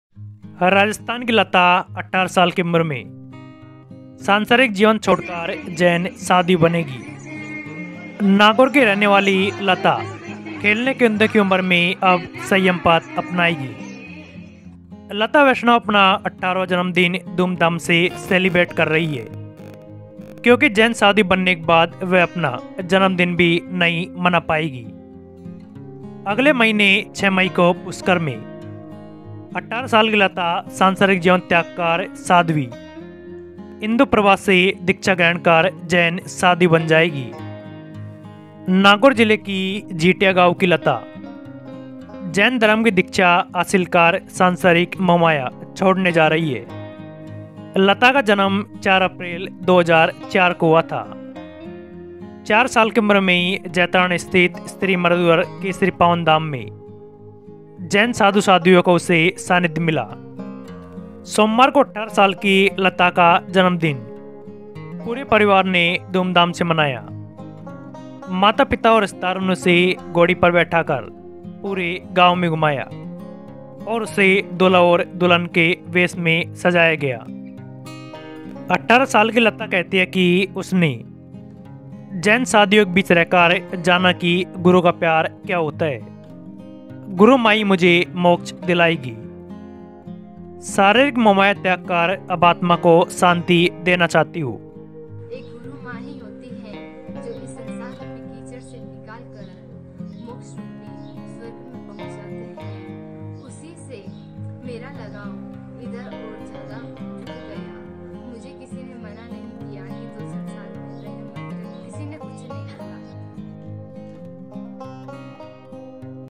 राजस्थान में नागौर की 18 साल की बेटी बनेगी जैन साध्वी , दूल्हा-दुल्हन बनकर यूं मनाया अंतिम बर्थडे
नागौर की 18 साल की बेटी बनेगी जैन साध्वी , दूल्हा-दुल्हन बनकर यूं मनाया अंतिम बर्थडे

राजस्थान न्यूज डेस्क, पढ़ने, लिखने, खेलने और कूदने के लिए 18 वर्ष। सपना सच हो जाता है, लेकिन लता ने सांसारिक सुख हमेशा के लिए छोड़ने का फैसला किया है। लता वैष्णव अपना 18वां जन्मदिन धूमधाम से मना रही हैं, क्योंकि वह जैन नन बनने के बाद अपना जन्मदिन नहीं मना पाएंगी। लता अगले महीने 6 मई को पुष्कर में अपना सांसारिक जीवन त्याग कर साध्वी इंदुप्रभा से दीक्षा लेकर जैन साध्वी बनेंगी।

नागौर जिले के खिंतिया गांव की लता जैन धर्म में दीक्षा लेकर अपने सांसारिक प्रेम का परित्याग करने जा रही हैं। लता का जन्म 4 अप्रैल 2004 को हुआ था। 4 साल की उम्र से ही जैतारण के श्रीमरुधर केसरी पावनधाम में जैन भिक्षुओं और ननों से उनका परिचय हो गया। लता ने भी दस बजे तक वहीं पढ़ाई की।

सोमवार को पूरे परिवार ने लता का जन्मदिन मनाया। माता-पिता और रिश्तेदार घोड़े पर सवार होकर उसे गांव के चारों ओर घुमाते थे। वह दूल्हा-दुल्हन के रूप में तैयार हुई थी। उनकी हर इच्छा पूरी हुई। ग्रामीणों ने लता का स्वागत किया और उन्हें बहुत सम्मान दिया। लता बेहद खुश दिख रही हैं। घोड़े पर सवार होकर शोभायात्रा निकाली गई, फिर पूरे गांव ने अपने काम की जगह छोड़ दी और जुलूस निकाला।

नागौर न्यूज

Nagaur News Lta Jain Sadhvi News today Rajasthan
Lata Jain Sadhvi Nagaur News Rajasthan